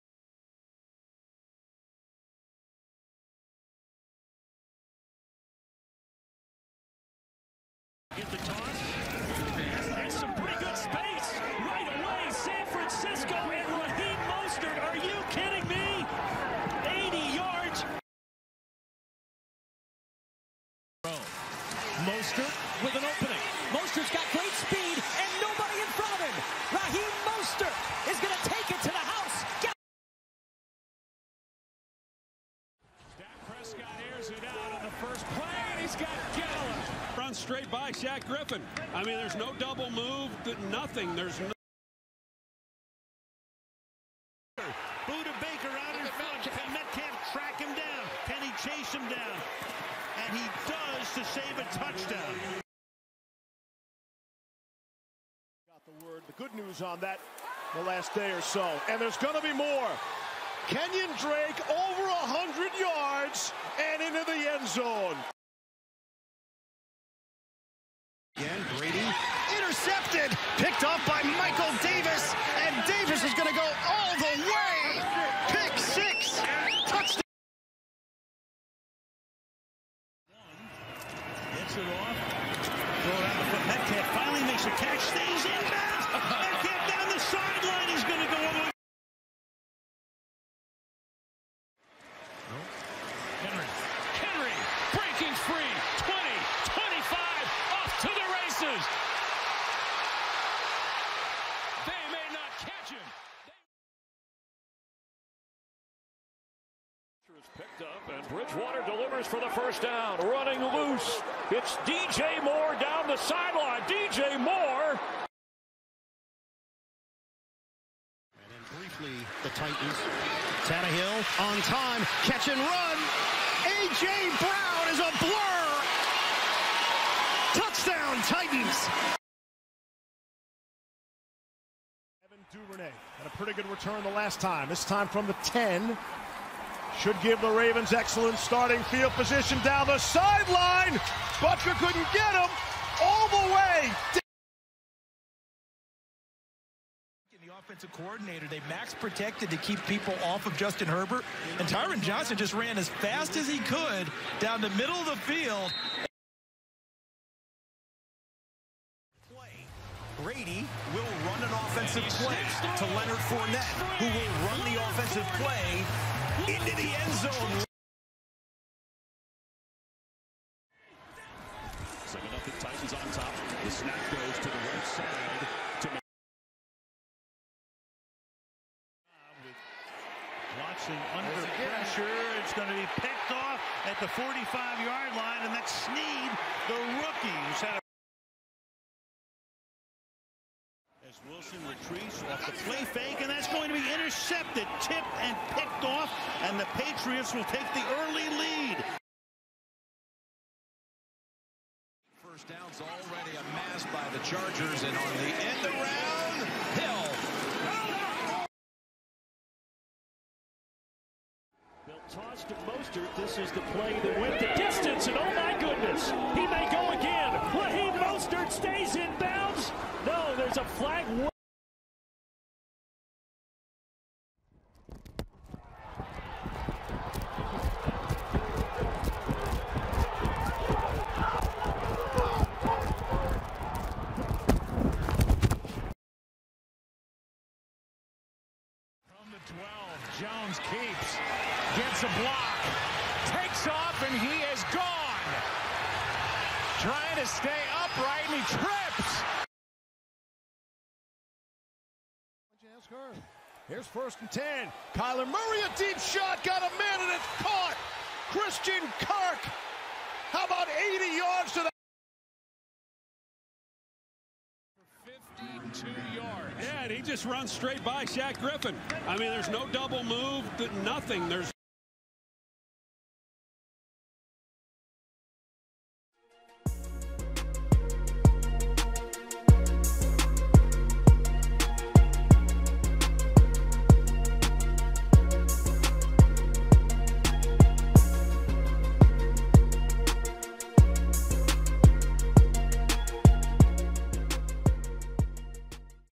NFL Fastest Players of 2020 (So Far)
0:08 - Raheem Mostert
0:20 - Raheem Mostert
0:32 - Shaquill Griffin
0:44 - D.K. Metcalf
0:55 - Kenyan Drake
1:08 - Michael Davis
1:20 - D.K. Metcalf
1:31 - Derrick Henry
1:46 - D.J. Moore
1:58 - A.J. Brown
2:12 - Devin Duvernay
2:27 - Tyron Johnson
2:39 - Leonard Fournette
2:51 - Isaiah McKenzie
2:59 - L'Jarius Sneed
3:11 - Devin McCourty
3:20 - Tyreek Hill
3:31 - Raheem Mostert
3:44 - Budda Baker
3:59 - Daniel Jones
4:10 - Christian Kirk
4:24 - Michael Gallup